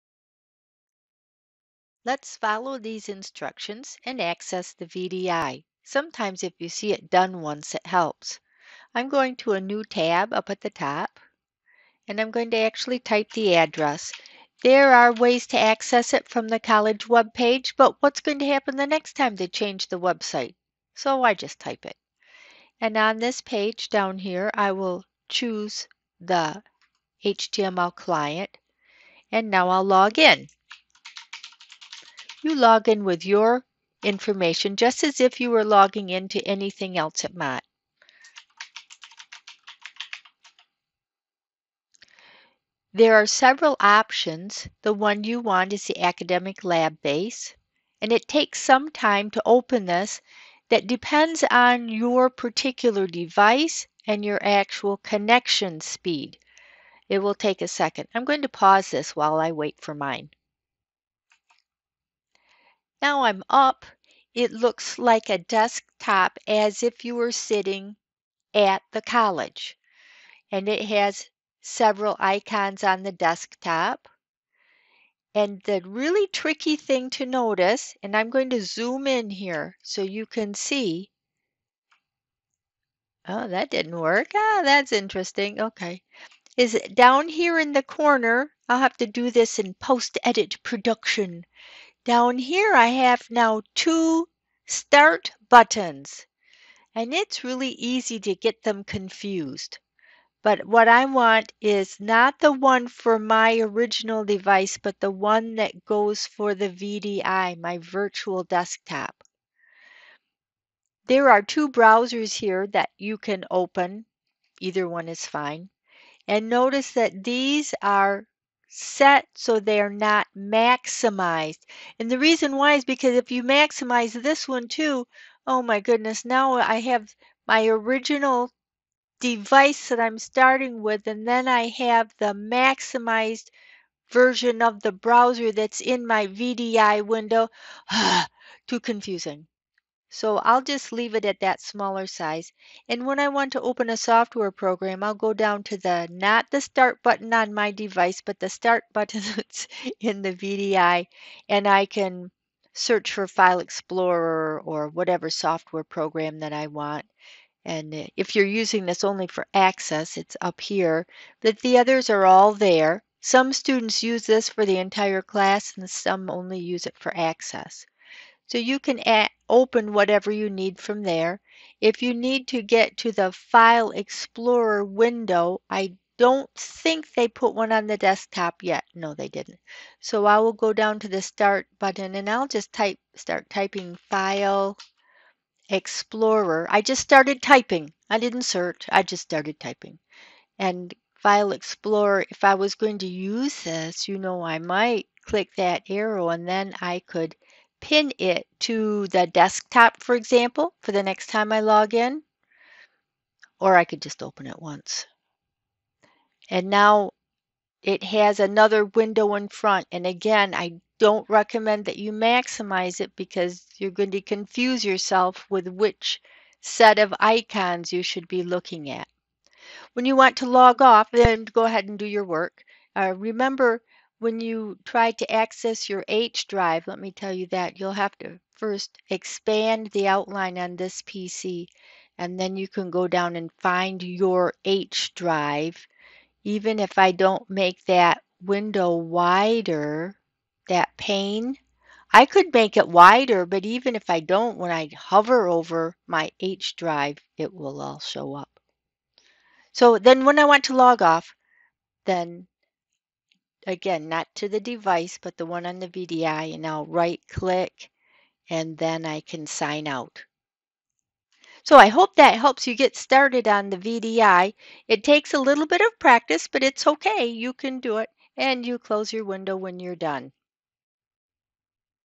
VDI Login Demo
(4:32) See a demonstration of how to log into MCC's Virtual Desktop Interface.
NOTE: (After January 2021) The couple of seconds near the end talking about the h:\ drive do not apply anymore.
View this video to see how to save and transfer files between your local computer and the VDI.  "How NOT to Lose Files in the VDI." (6:03)